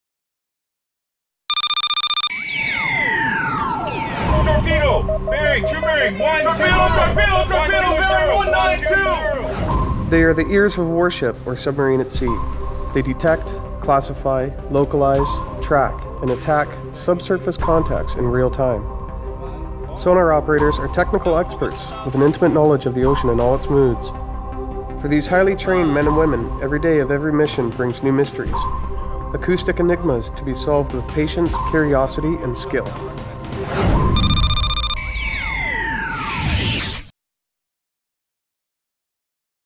Sonar Operator
Sonar Operators compile and analyze acoustic intelligence information. The Sonar Operator may be employed in three distinct areas of Anti-Submarine Warfare operations: surface ships, submarines, or Integrated Undersea Surveillance.

For more information on this career or any other career with the Canadian Armed Forces,